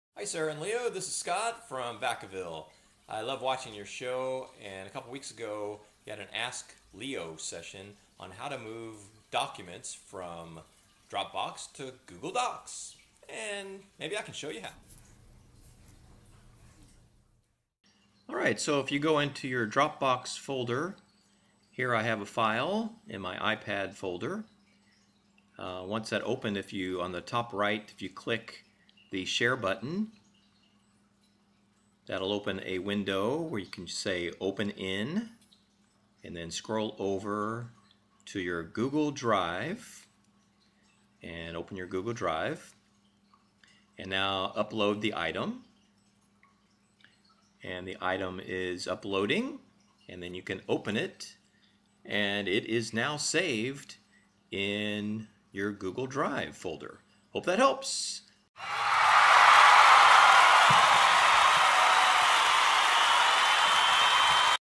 How to Move Dropbox Files to Google Drive
short video to show you how to move files from your Dropbox folder to your Google Drive folder on your iPad